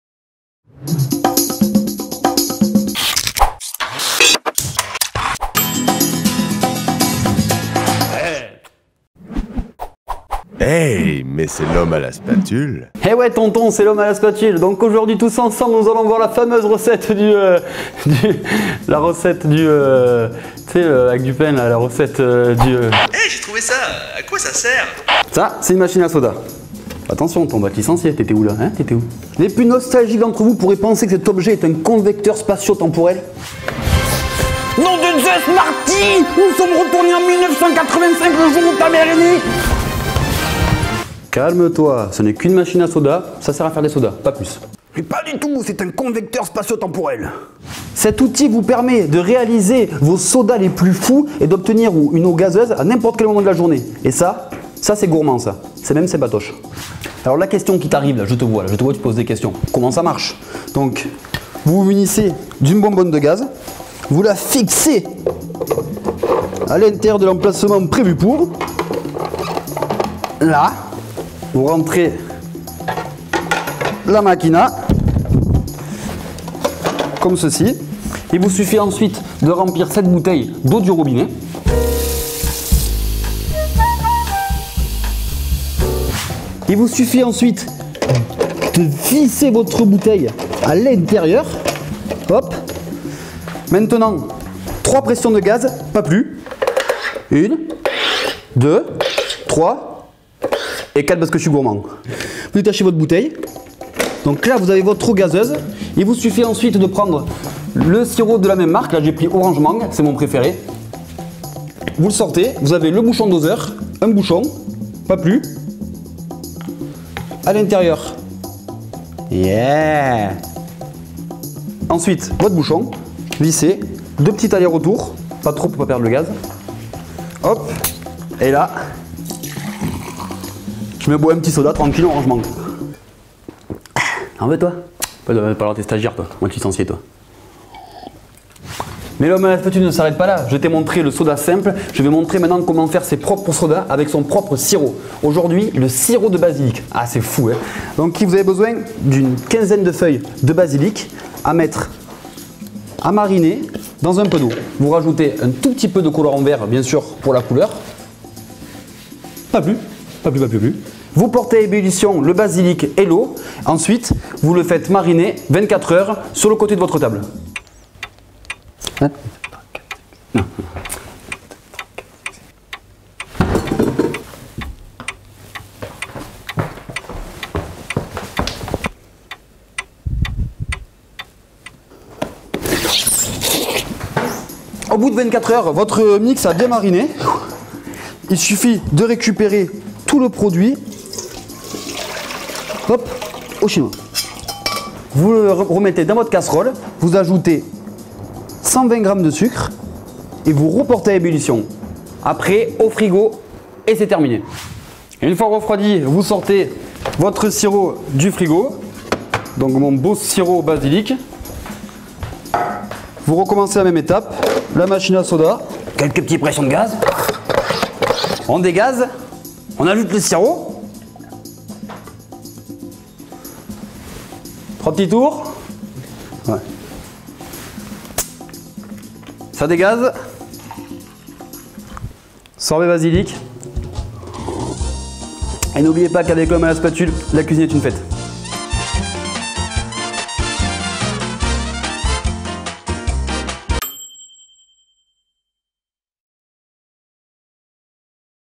Recette de Soda Maison au Basilic
Toc (Trouble Obsessionnel Culinaire) et l'Homme à la Spatule vous apprennent à réaliser une recette de soda maison parfumé au sirop de basilic grâce à une machine à eau gazeuse Soda Stream.